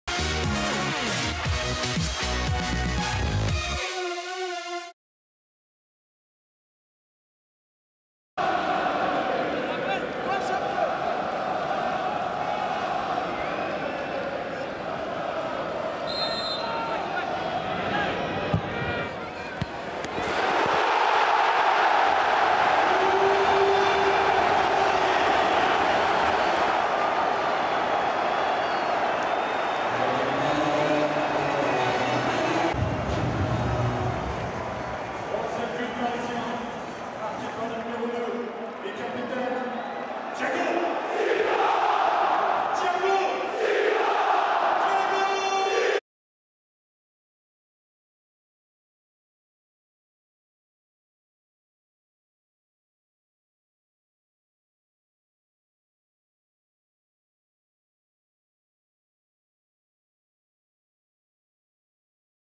But Thiago SILVA (74') / Paris Saint-Germain - Toulouse FC (3-1) - (PSG - TFC) / 2014-15
Paris Saint-Germain - Toulouse FC (3-1) - But de Thiago SILVA (74'). Tous les buts de Paris Saint-Germain - Toulouse FC en vidéo.

Parc des Princes - samedi 21 février 2015

Buteurs : Adrien RABIOT (27' - Paris Saint-Germain) / Adrien RABIOT (48' - Paris Saint-Germain) / Wissam BEN YEDDER (51' - Toulouse FC) / Thiago SILVA (74' - Paris Saint-Germain)